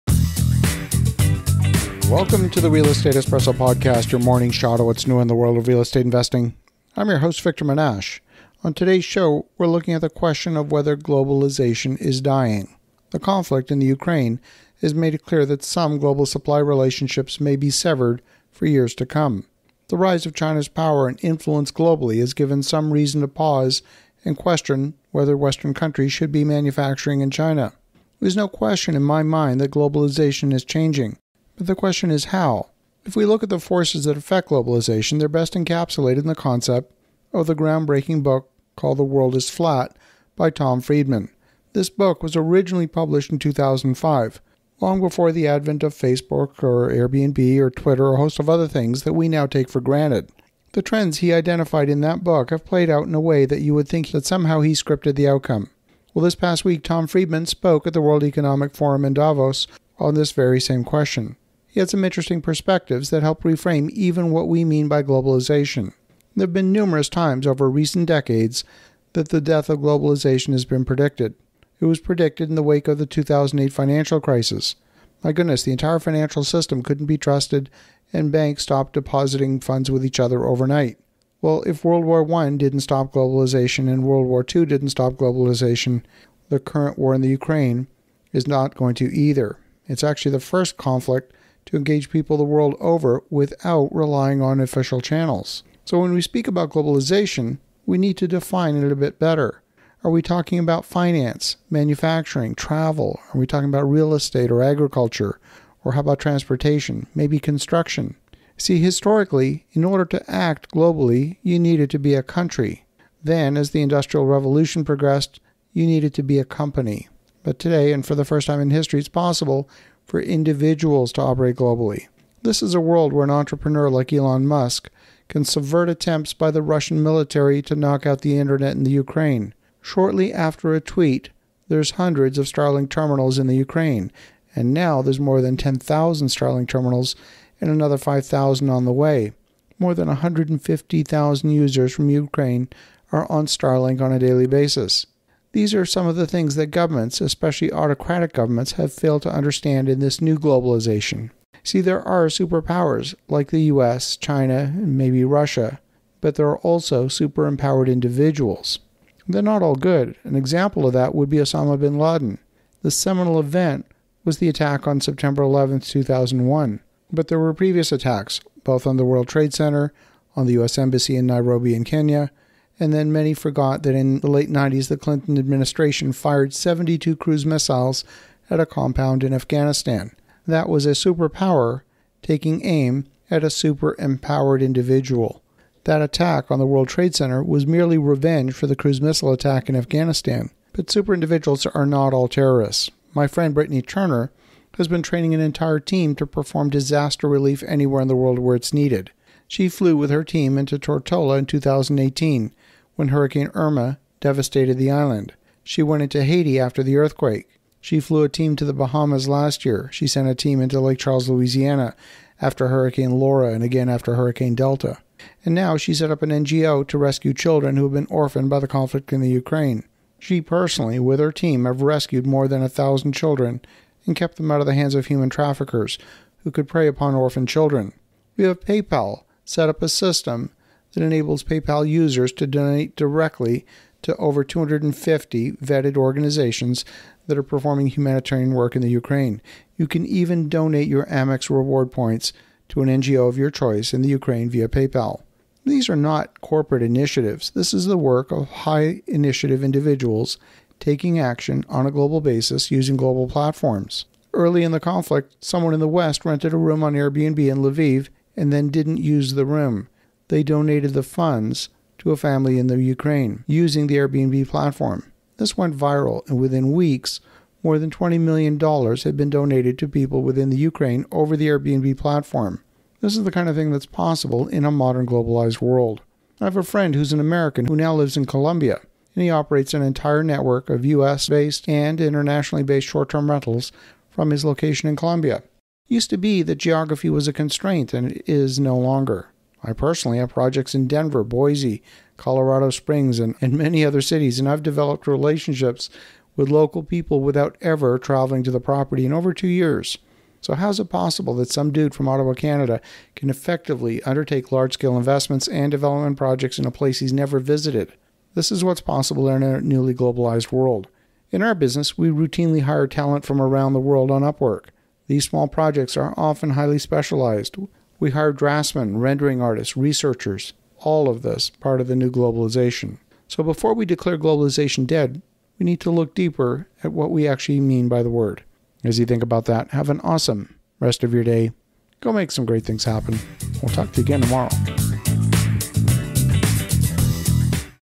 Is Globalization Dying?
On today’s show we are looking at the question of whether globalization is dead. The conflict in the Ukraine has made it clear that some global supply relationships may be severed for years to come. The rise of China’s power and influence globally has given some reason to pause and question whether western countries should be manufacturing in China.
There is no question in my mind that globalization is changing, but the question is how?
If we look at the forces that affect globalization, they are best encapsulated in the concepts of the ground-breaking book “The World is Flat” by Tom Friedman. This book was originally published in 2005 before the advent of Facebook, or AirBnb, or Twitter or a host of things that we now take for granted. The trends he identified in that book have played out in a way that you would have think he scripted the outcome.
When we speak about globalization, we need to define it a bit better. Are we talking about finance, manufacturing, travel, real estate, agriculture, transportation, construction.
Historically, to act globally, you needed to be a country. Then as the industrial revolution progressed, you needed to be a company. Today, for the first time in history, it is possible for individuals to operate globally.
This a world where an entrepreneur like Elon Musk can subvert attempts by the Russian military to knock out the Internet in the Ukraine. Shortly after a tweet, there are hundreds of Starlink terminals in the Ukraine. Now there are more than 10,000 Starlink terminals and another 5,000 are on the way. More than 150,000 users from Ukraine are on Starlink on a daily basis.